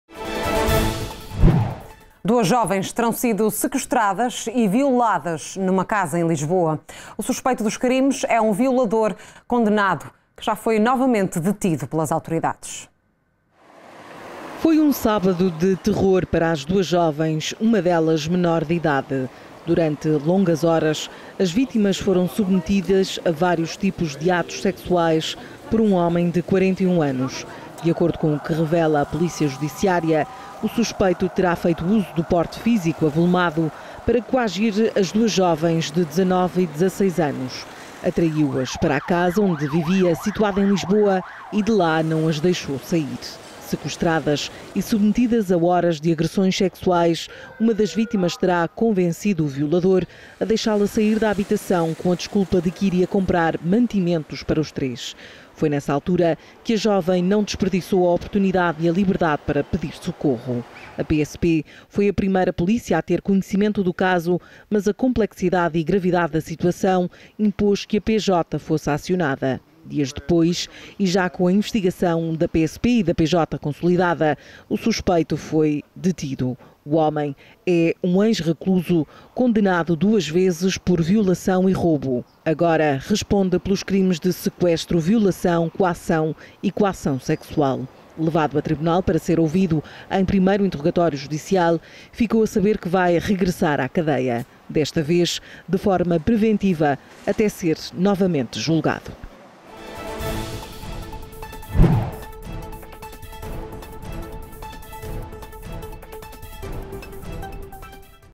Sequestradas e violadas em Lisboa
Duas jovens terão sido sequestradas e violadas numa casa, em Lisboa. O suspeito dos crimes é um violador condenado que já foi detido novamente pelas autoridades.